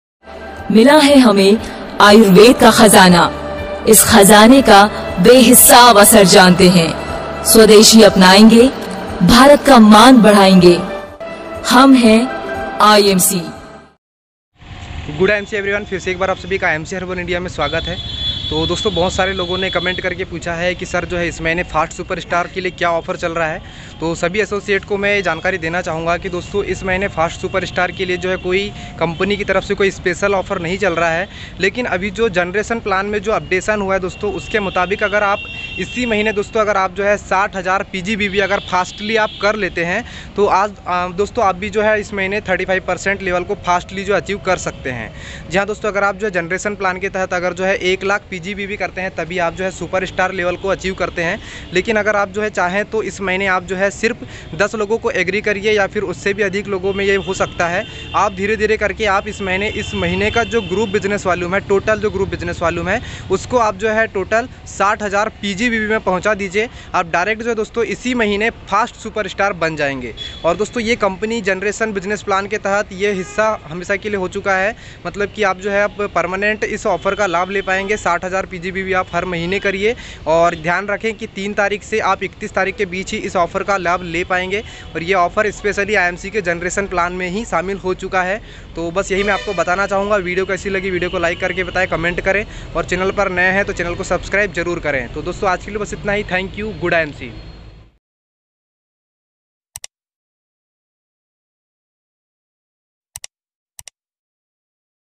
दिसंबर 2020 में IMC में सुपर स्टार के लिए कौन-सा आफर है? Fast 35% कैसे करें।
How to make Fast Super star in IMC DECEMBER MONTH.